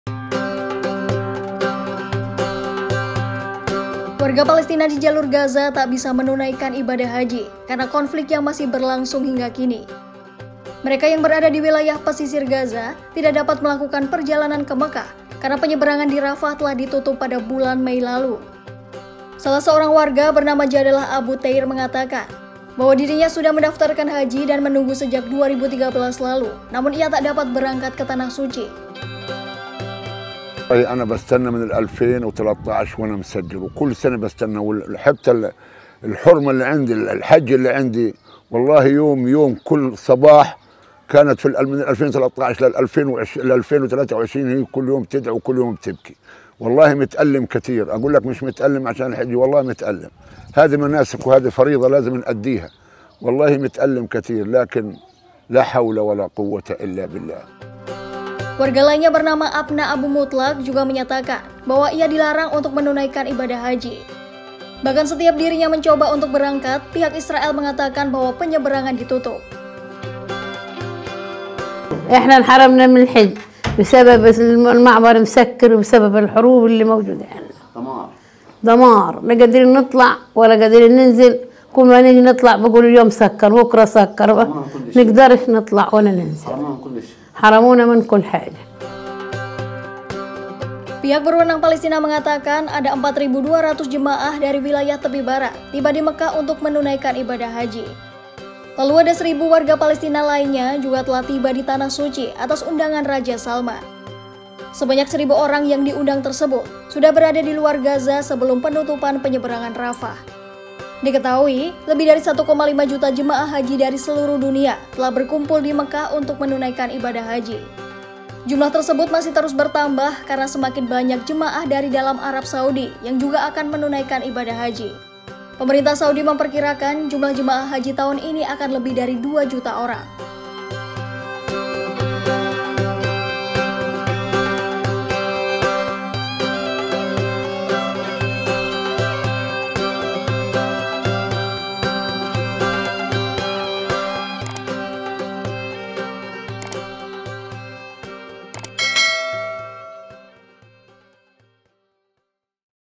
Cerita Warga Palestina Tak Bisa Tunaikan Haji karena Perang Israel-Hamas
Warga Palestina tidak bisa menjalankan Ibadah haji tahun ini karena konflik yang terus terjadi.

Warga Palestina di wilayah pesisir Gaza, tidak dapat melakukan perjalanan ke Mekah untuk menunaikan ibadah haji tahun ini, karena penutupan penyeberangan Rafah pada bulan Mei lalu.

Namun pihak berwenang Palestina mengatakan, ada 4.200 jamaah dari wilayah Tepi Barat telah tiba di Mekah untuk menunaikan ibadah haji.

Selain itu, menurut pihak berwenang Saudi, ada 1.000 warga Palestina telah tiba untuk menunaikan ibadah haji atas undangan Raja Salman.

Penulis Naskah: Hanindiya Dwi Lestari
Narator: Hanindiya Dwi Lestari
Video Editor: Dina Rahmawati
Produser: Adisty Safitri